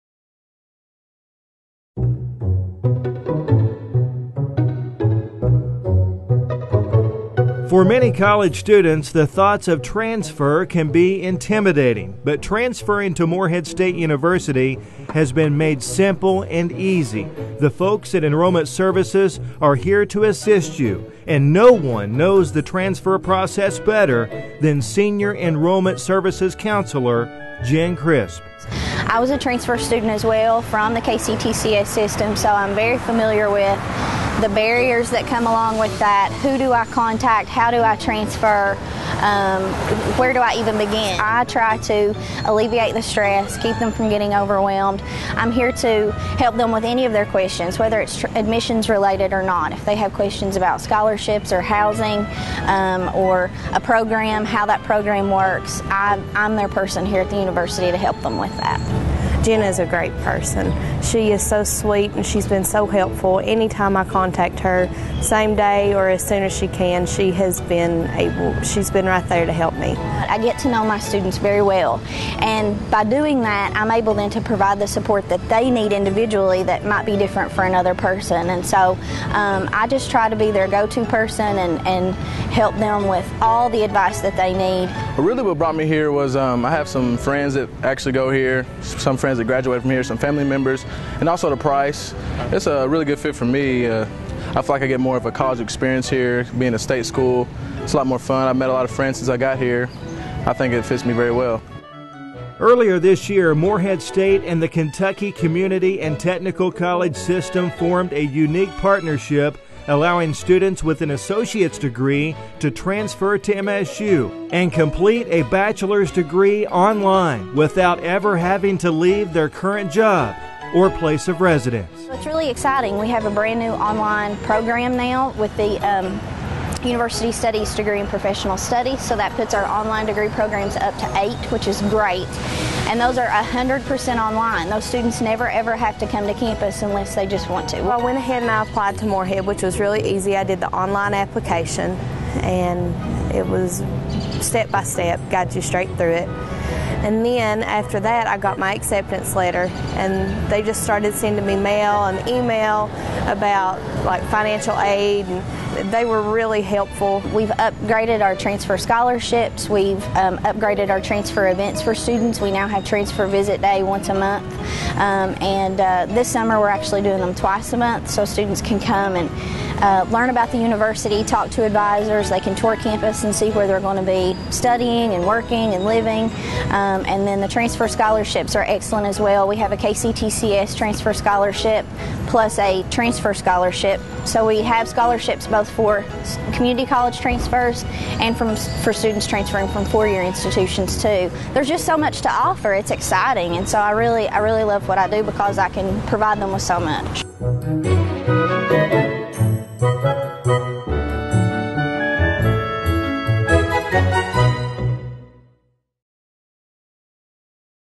Transferring to MSU?
Considering a transfer to Morehead State University?  Senior Enrollment Services Counselor Jen Crisp makes the process simple.